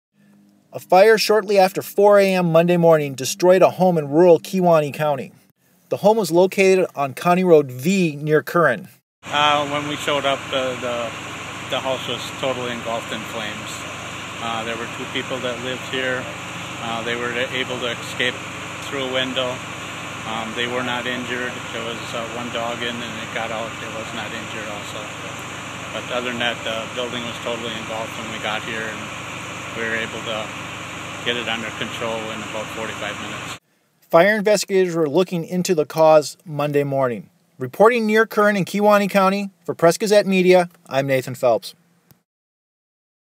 Fire destroys Curran house
Fire destroyed a home in Curran, just east of Denmark in southwestern Kewaunee County, early Monday morning. No injuries were reported.